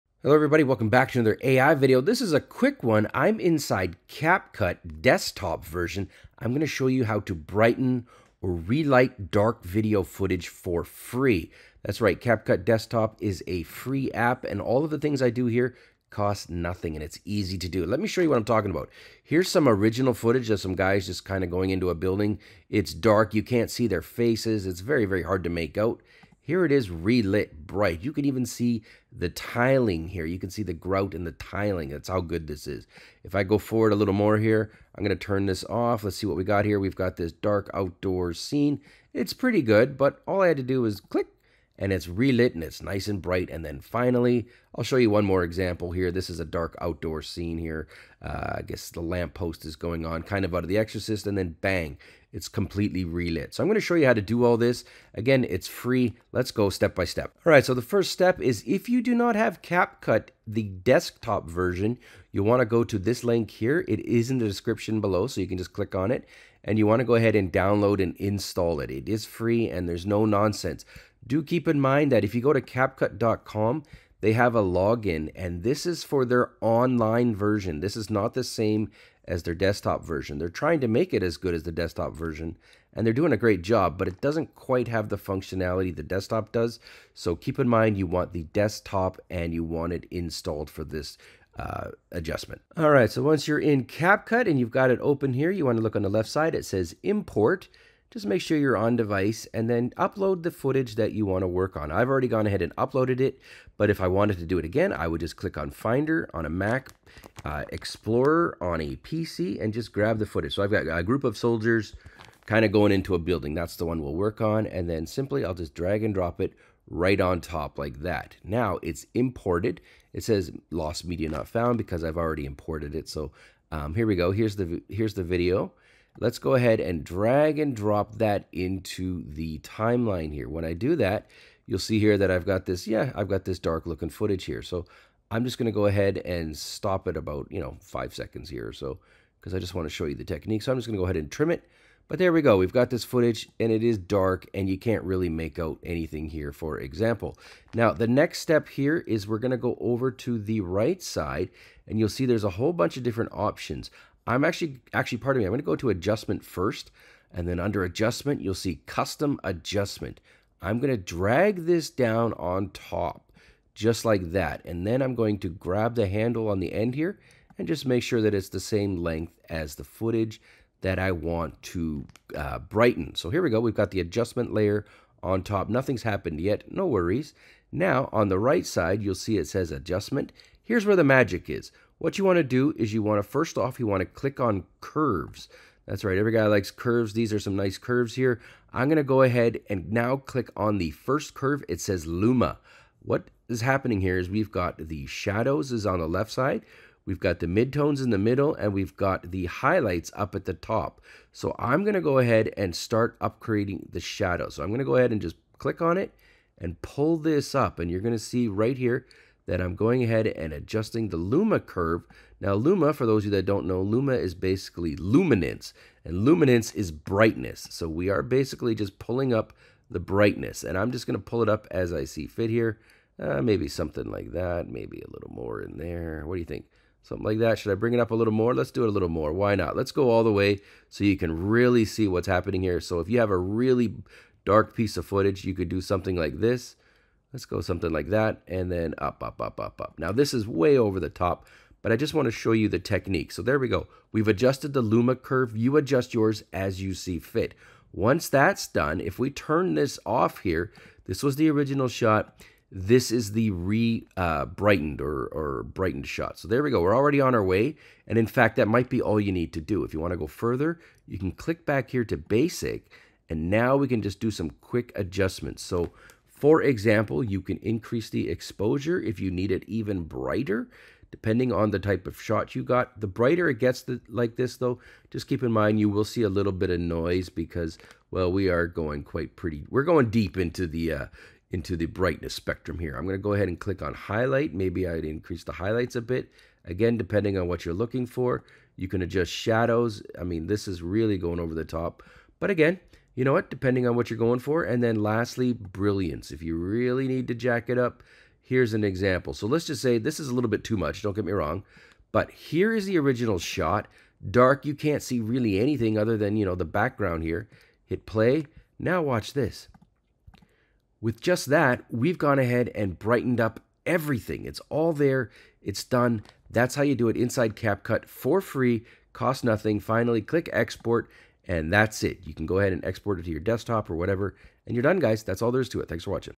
How to Brighten And Lighten Dark Video Footage Using CapCut Desktop - Step by Step Guide
Welcome to our quick and informative tutorial on how to enhance your dark video footage for FREE using CapCut Desktop. Whether you're struggling with dimly lit scenes or just want to improve the overall brightness and visibility of your videos, this guide is for you. Let’s dive into transforming your videos with easy, cost-free adjustments!

Timestamps:
0:00 - Introduction to Brightening Dark Video Footage in CapCut
0:19 - Demonstrating the Original Dark Footage
0:35 - First Look at the Brightened Footage
0:53 - Step-by-Step Guide Starts
1:08 - Downloading CapCut Desktop Version
1:27 - Important Note on Desktop vs. Online Version
1:32 - Importing Your Footage into CapCut
2:10 - Trimming and Preparing the Footage
2:26 - Applying Custom Adjustments
3:11 - Adjusting Luma Curves for Brightness
4:03 - Final Adjustments and Enhancements
5:24 - Exporting Your Enhanced Video
5:26 - Conclusion and Thanks for Watching

What You Will Learn:

The difference between CapCut's online and desktop versions.
Simple steps to import and prepare your footage.

How to use custom adjustments and Luma curves to brighten your video effectively.

Additional tips on enhancing exposure, highlights, shadows, and brilliance for the perfect finish.

Why CapCut Desktop?

CapCut Desktop offers a powerful, free platform for video editing without the complexity or cost of professional software. It's user-friendly, making it accessible for beginners and experienced editors alike.

Remember: This tutorial is all about making those adjustments free of charge, providing you with the knowledge to significantly improve your video's quality with a few clicks.

We hope this tutorial empowers you to bring your dark footage to light, showcasing your moments clearly and beautifully.